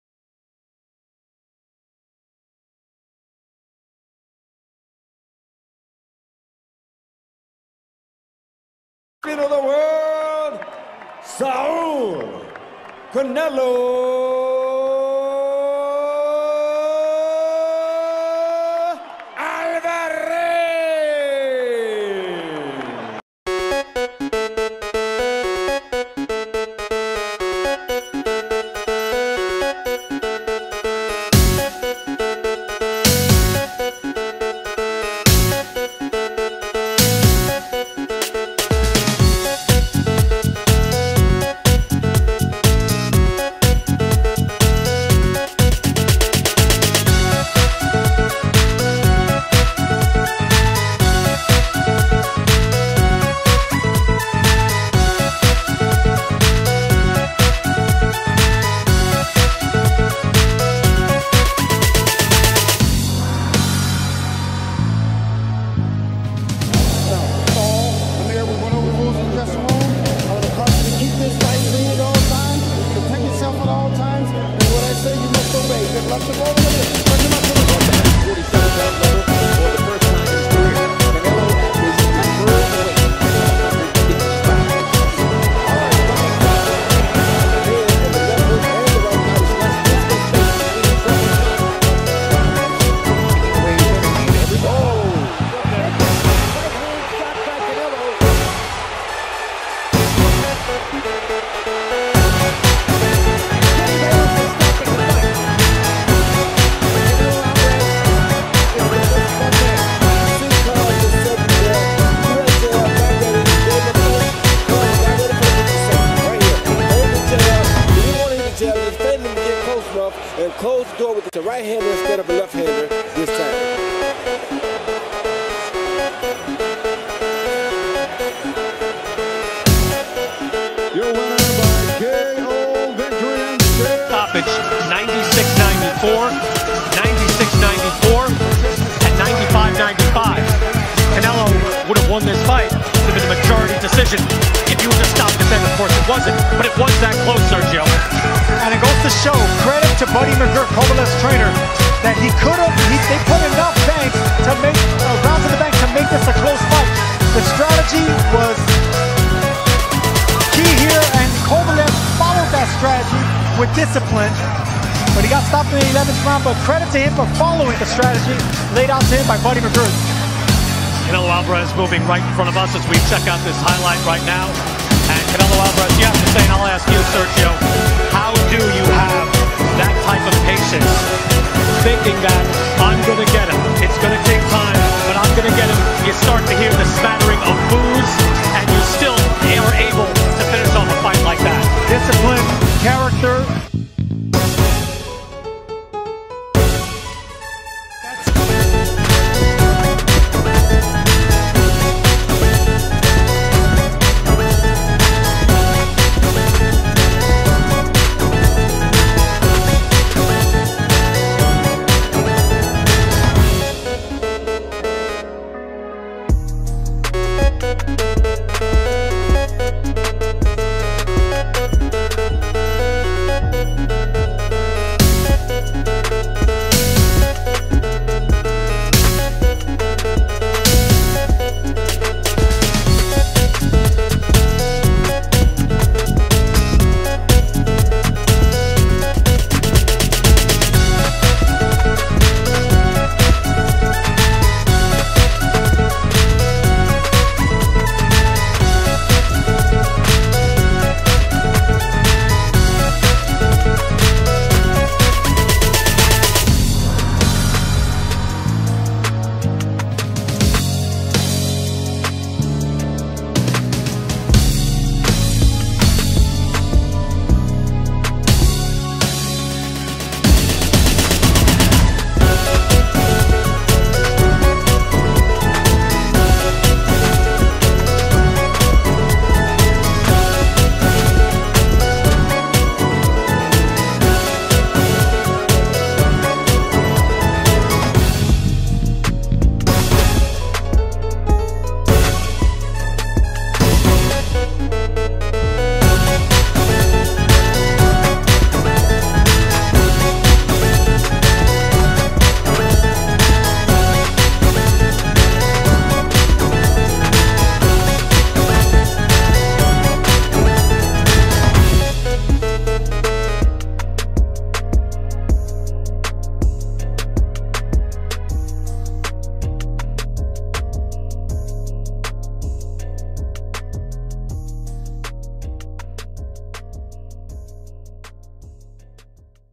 Canelo Alvarez vs Calum Smith OFFICIAL FIFGHT PROMO
I am a Boxing fan, i just want to share some of the best highlights of the both fighters before they clash this coming DECEMBER 19, 2020.